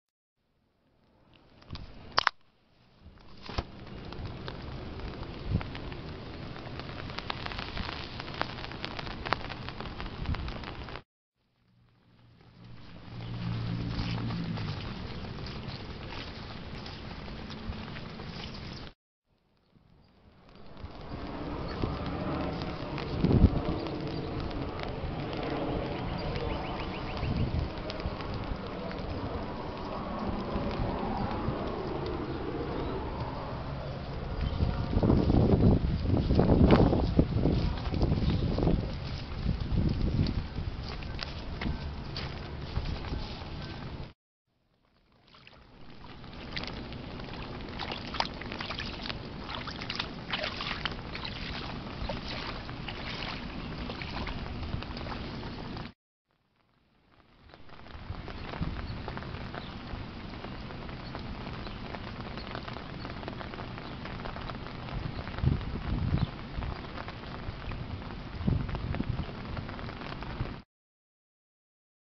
Gentle Rain Shower woman walking in the with boots galosh wellies
A quiet and gentle rain shower. I like these better than the thunder and lightning and beat down rain showers.